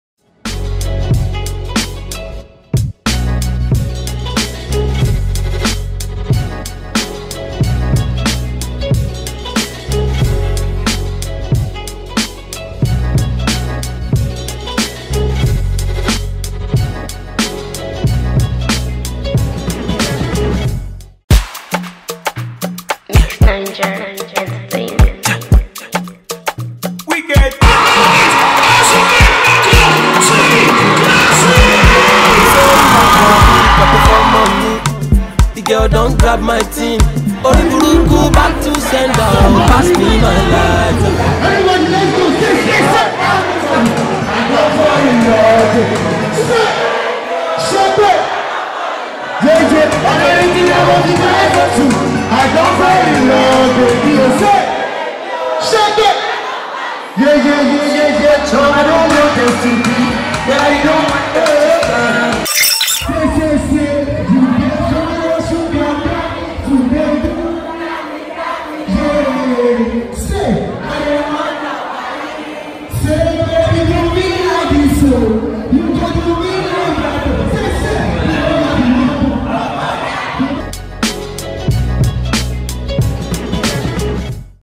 T CLASSIC LEADCITY SHOW ONK
Fast rising Nigeria pop artist T Classic is now touring round all University in Nigeria with his energetic performance.You can't but love him because is one of the next rated .We have lots for your views, just kindly Subscribe to our youtube channel ONKTV to see more of Entertaining videos first..